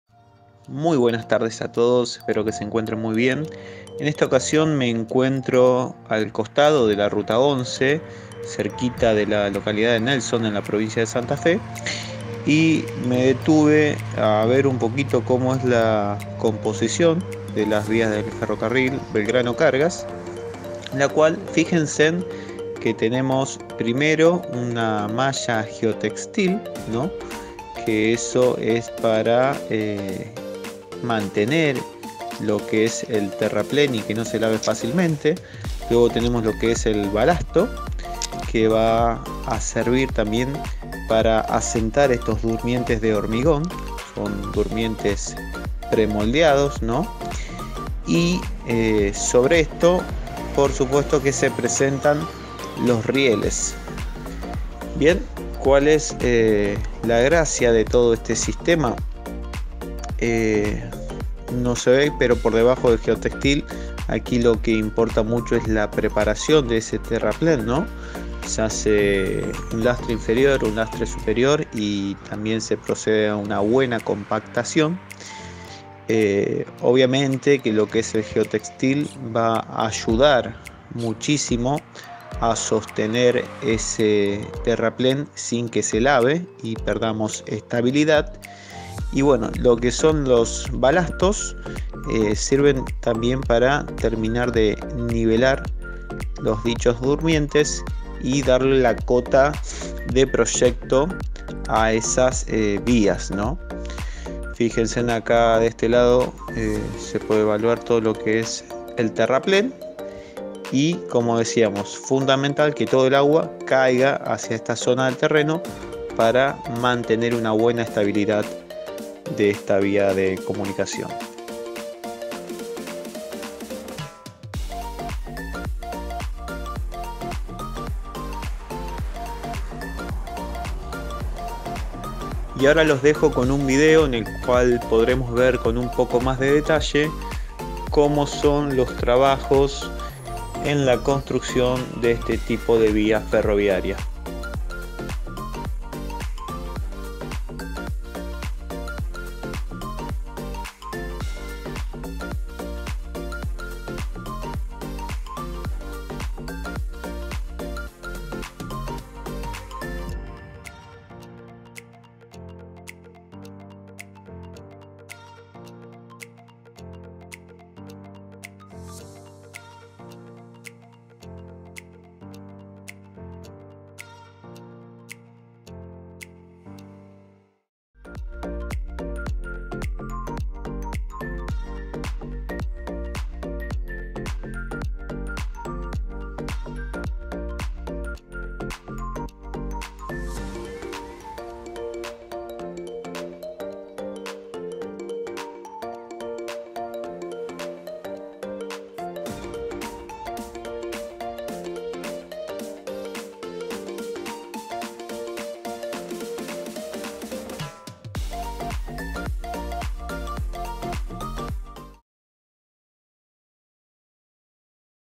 VÍAS TREN BELGRANO CARGAS | Análisis
En este video analizamos brevemente como están conformadas las Vías del Tren Belgrano Cargas.